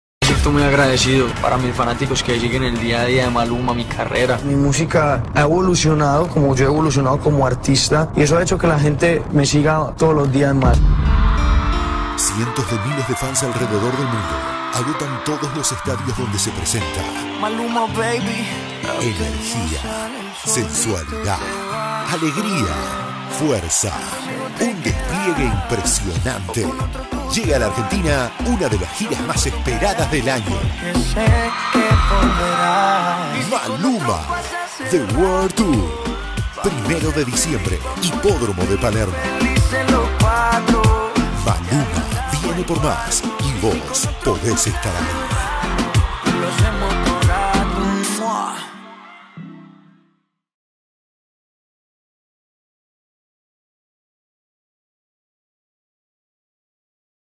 🇦🇷 Argentina! Mañana a la venta las primeras fechas de la gira! 01|12 BUENOS AIRES
@maluma🇦🇷 Argentina! Mañana a la venta las primeras fechas de la gira!
01|12 BUENOS AIRES, Hipodromo de Palermo
26|11 ROSARIO, Hipodromo de Rosario
25|11 CÓRDOBA, Orfeo Superdomo
03|09 SAN JUAN, Estadio Abierto Parque de Mayo
Pronto anunciaremos más conciertos! Atent@s!